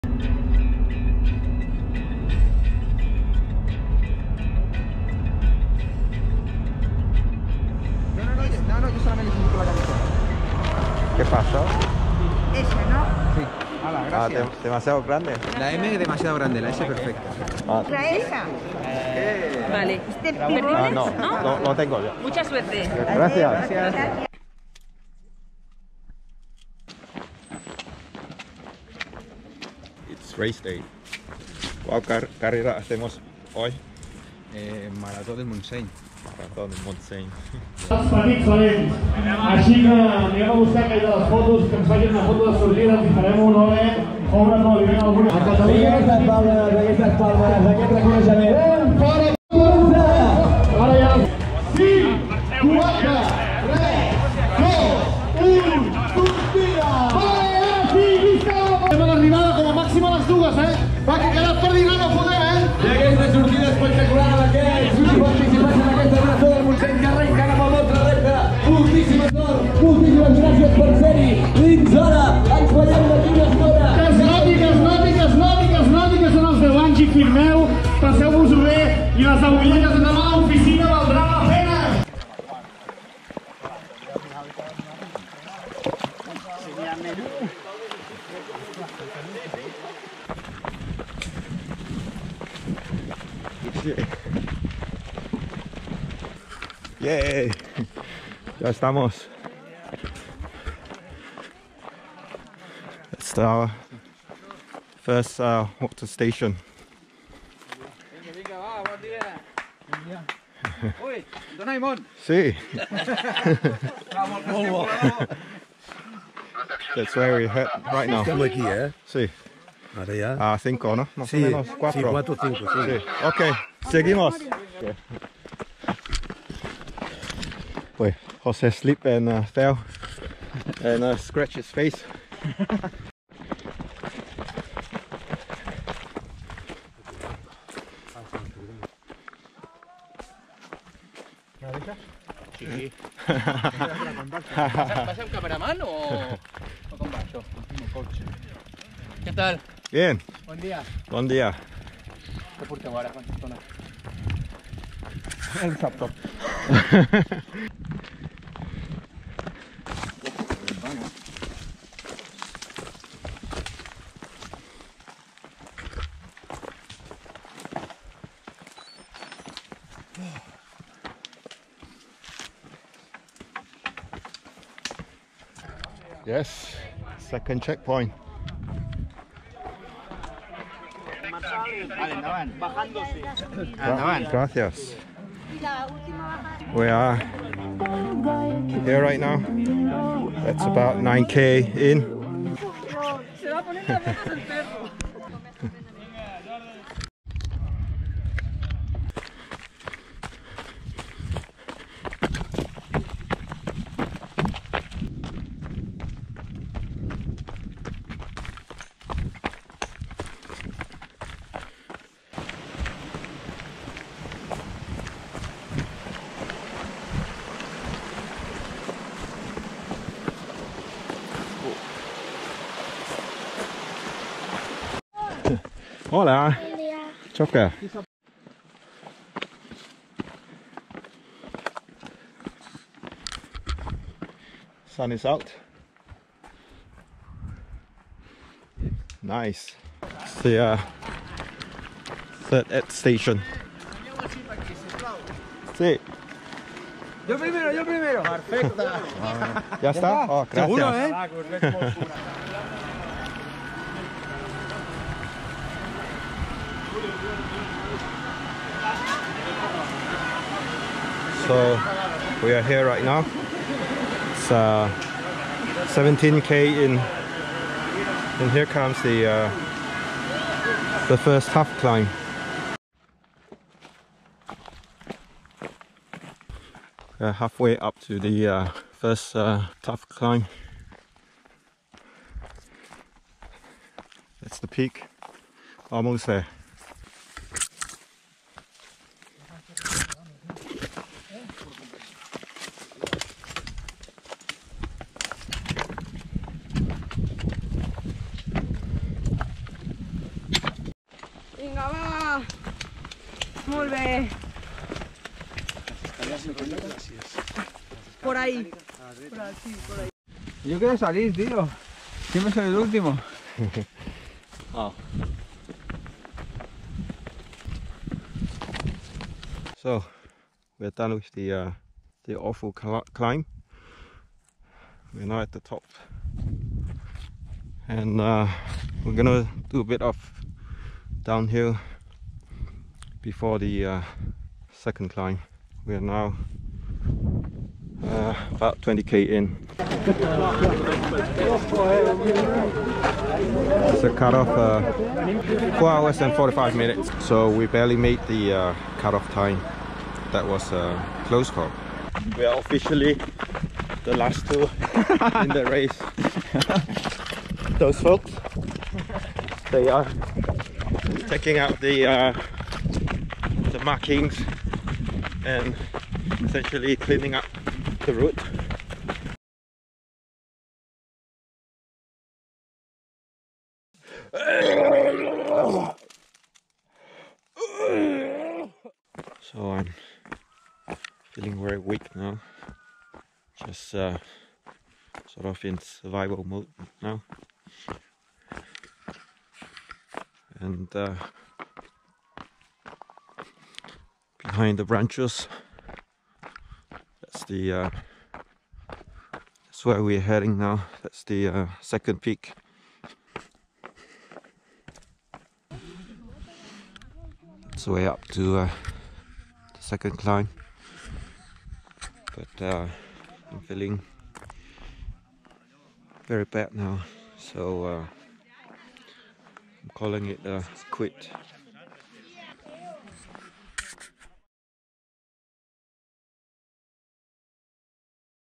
Marató del Montseny 2021: How NOT to trail running
My second Marató del Montseny (with Jose) and ended up badly :(